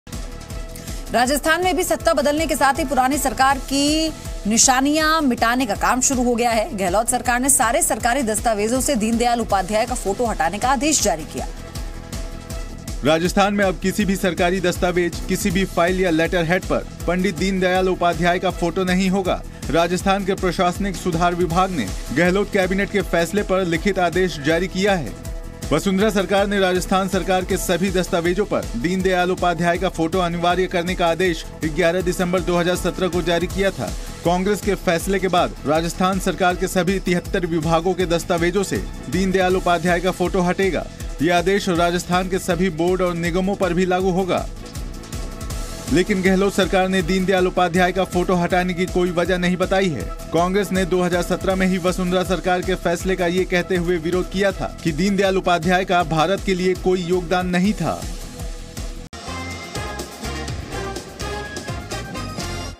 Morning Breaking: Rajasthan govt orders removal of Deendayal Upadhyaya’s photo from documents
The Rajasthan government has passed an order directing all departments, corporations, boards and self-government agencies in the state to remove the photo of Deendayal Upadhyaya from all official documents.